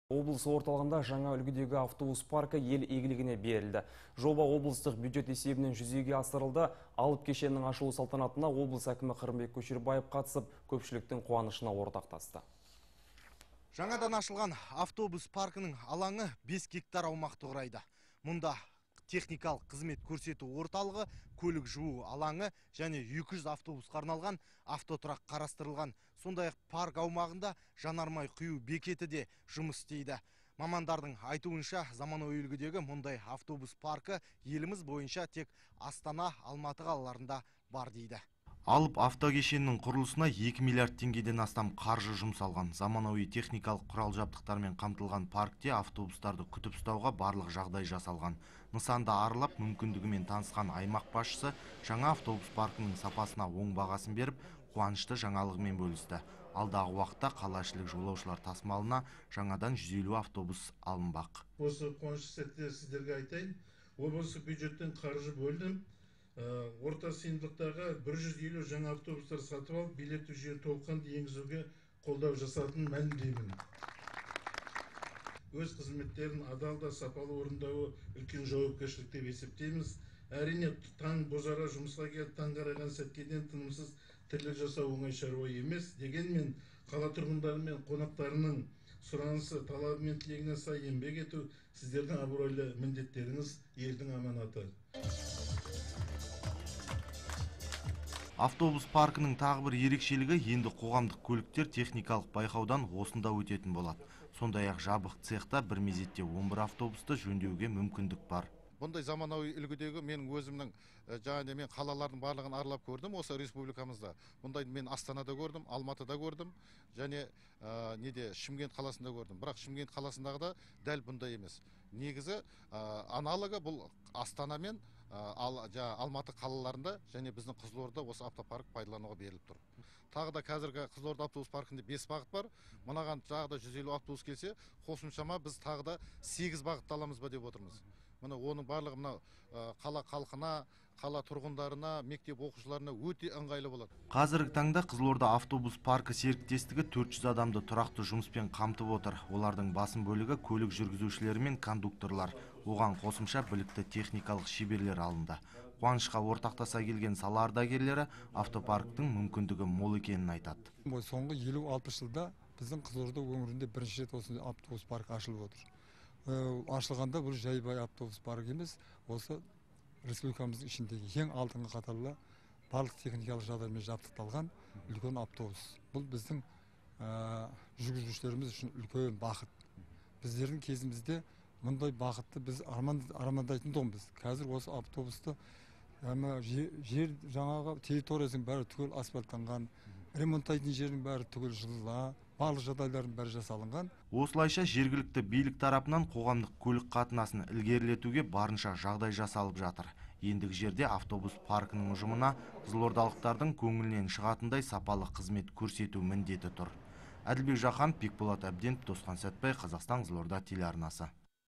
Жаңа автобус паркі сапалы қызметке жол ашпақ
Облыс орталығында жаңа үлгідегі автобус паркі ел игілігіне берілді. Жоба облыстық бюджет есебінен жүзеге асырылды. Алып кешеннің ашылу салтанатына облыс әкімі Қырымбек Көшербаев қатысып, көпшіліктің қуанышына ортақтасты.